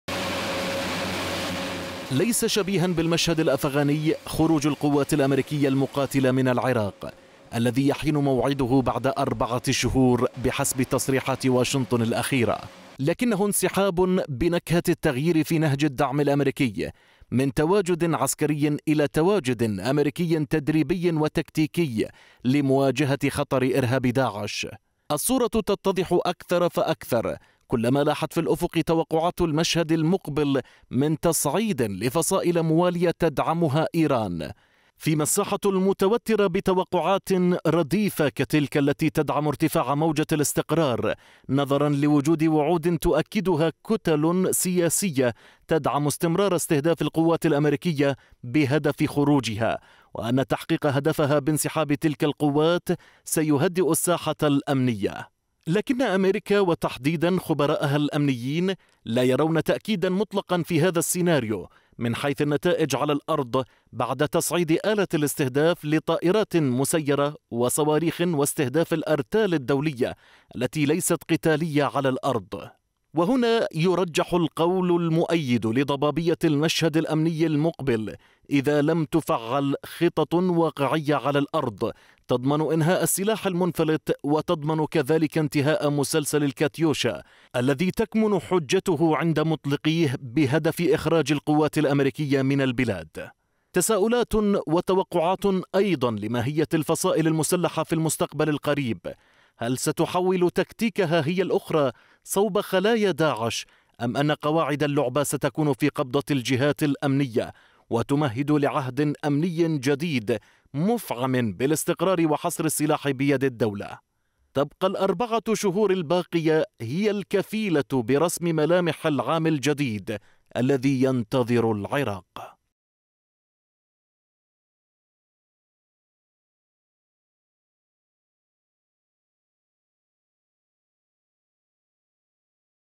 انسحاب القوات الامريكية من العراق .. بدء العد التنازلي والسيناريو المحتمل | تقرير
أربعة شهور تفصل العراق عن الانسحاب الأمريكي الذي أعلنت عنه اميركا مؤخرا، ما يفتح أبواب السيناريوهات المحتملة سيما مع وعود جهات داخلية بإيقاف السلاح المنفلت فيما واشنطن التي ترى وجهة نظر مختلفة على الساحة الأمنية العراقية.